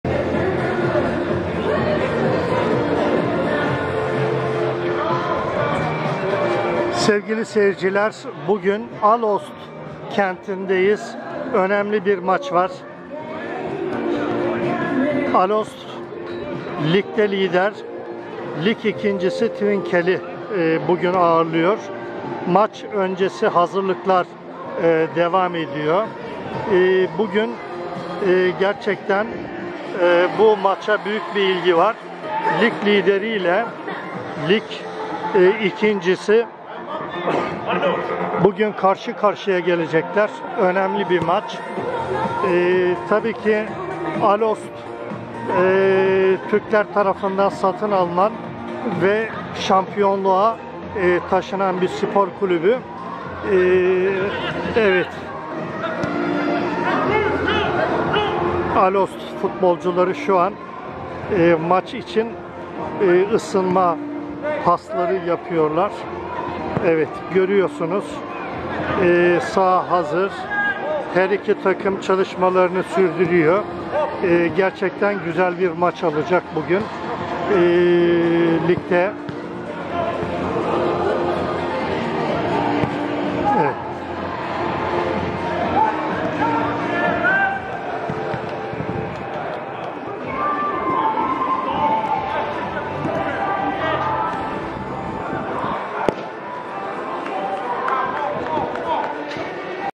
SC Eeendracht Allost,kendi evinde vurgun yedi:3-0
Ligi 25 maçta topladığı 46 puan ile lider götüren SC Eendracht Aalst  futbol takımı, lig ikincisi Dikkelvenne'ye beklenmedik şekilde 3-0 mağlup olması,hem teknik heyeti ve de yöneticileri olduğu kadar,taraftarlarını da şaşırttı,üzdü..
25 Maçta, 13 Galibiyet, 5 Yenilgi ve 7 beraberlik elde eden SC Eendracht Aalst futbol takımı rakip kalelere 38 gol atmayı başarırken, kendi kalesinde de 24 gol gördü.